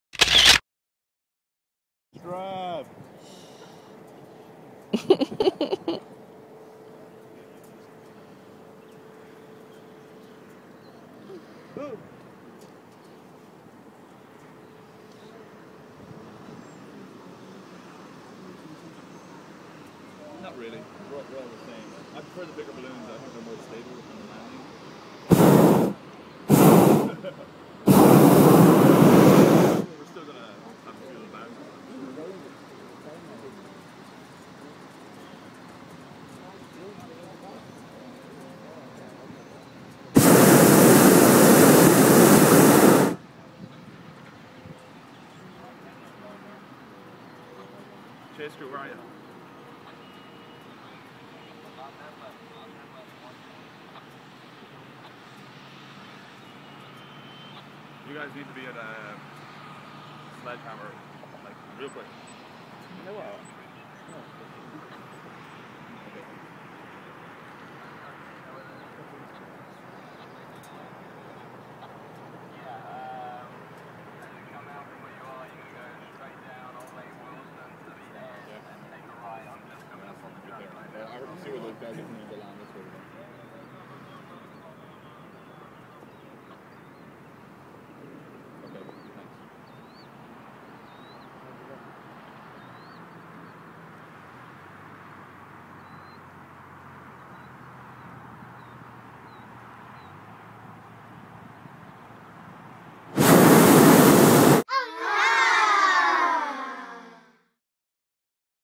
Reunion Resort - Flyover from a Hot Air Balloon
An amazing view of the beautiful Reunion Resort, which we just happened to fly over in an hot air balloon. You get a great view of their rooftop pool and the golf courses that surround it. The balloon operator was Damien from OrlandoBalloonRides.com -- such a smooth ride! This was my first time in a hot air balloon!